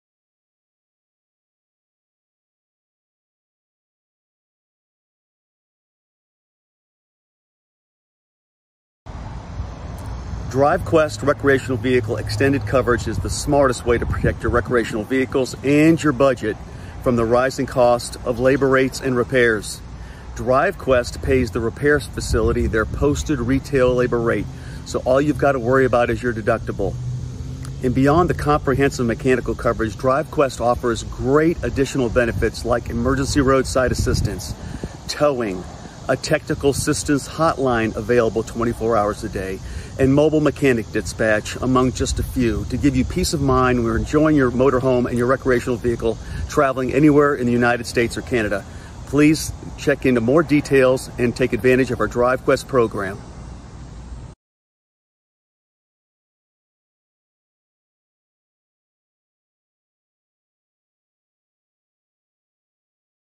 DriveQuest
FMCA is proud to bring it's members RV warranty protection. In the event of a mechanical breakdown DriveQuest's RV vehicle service protection offers a range of coverage choices so that you can get back on the road. Repairs can be completed at any licensed repair facility.